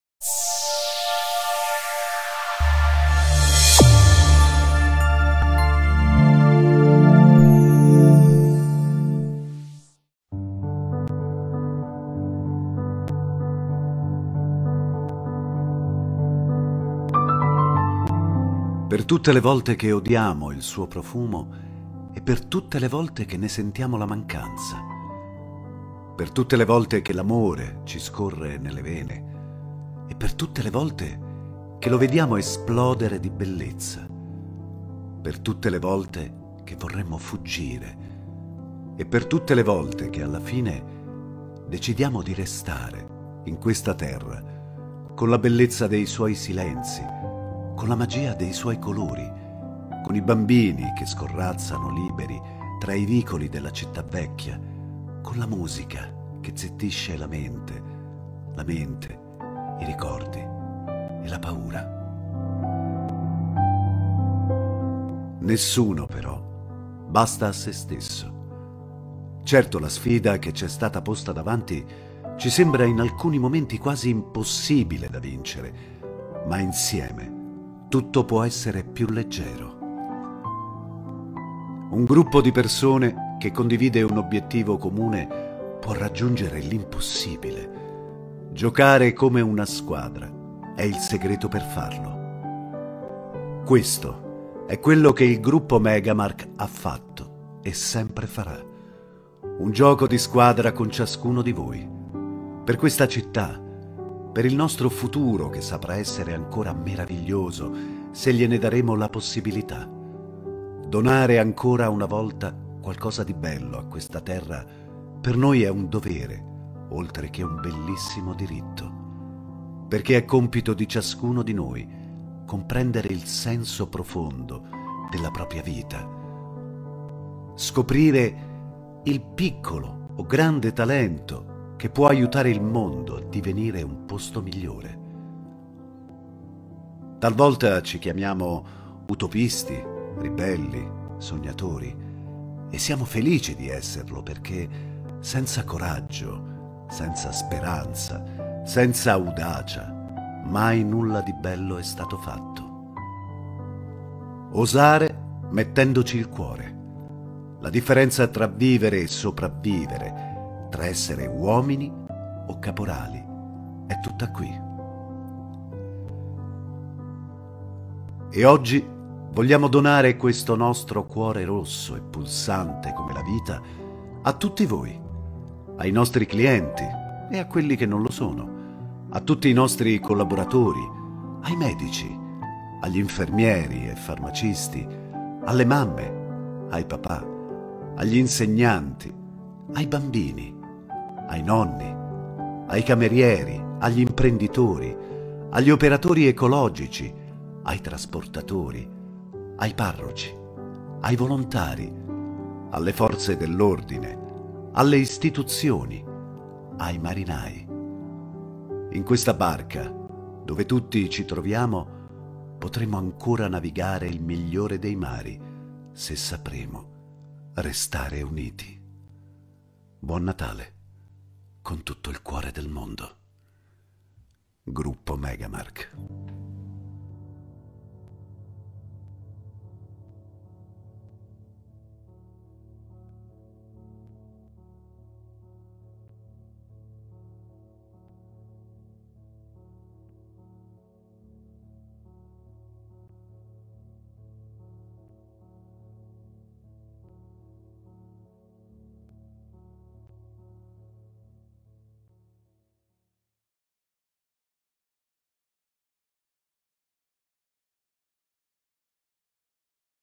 Bari - Christmas Luminarie Tree Walkthrough - The Build up
The waterfront of Bari lights up with a 27-meter tall Christmas tree, made by MarianoLight. The tree was donated to the city for the upcoming holidays by the Megamark foundation on the occasion of its 20 years anniversary. A walkthrough record tree made of traditional luminarie and rgb lights creates spectacular plays of light to brighten the apulian nights with Christmas traditional songs.
In this video the buld up of the tree.